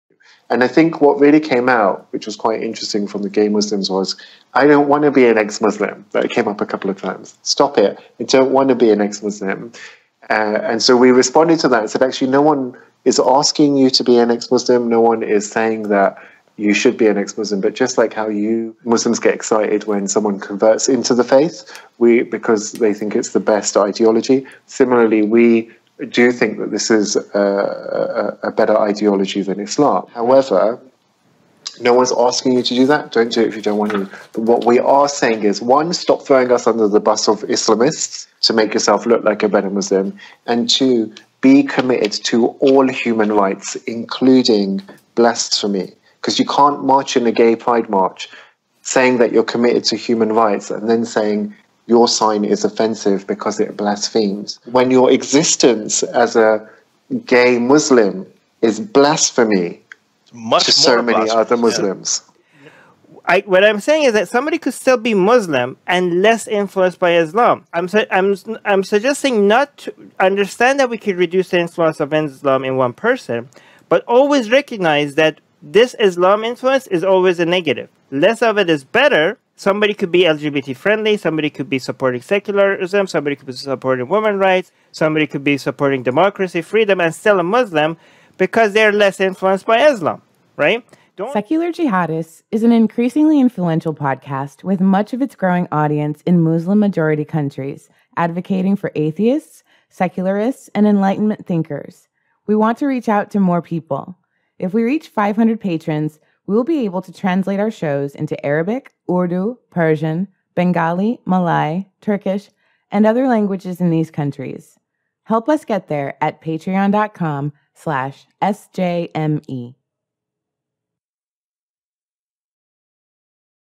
'Somebody could still be Muslim and less influenced by Islam'
We talk about gay Muslims and the influence of Islam with Jimmy Bangash.
Every day, we're seeing more courageous ex-Muslim and Muslim LGBTs rise up and smash the closet. From the reactionary parent protests in Birmingham to ex-Muslims marching at Pride in New York and London, this battle is just beginning. And our guest, the British-Pakistani ex-Muslim LGBT activist Jimmy Bangash, is right on the frontline. He joins us in this episode in an eye-opening conversation that's not to be missed. Check it out.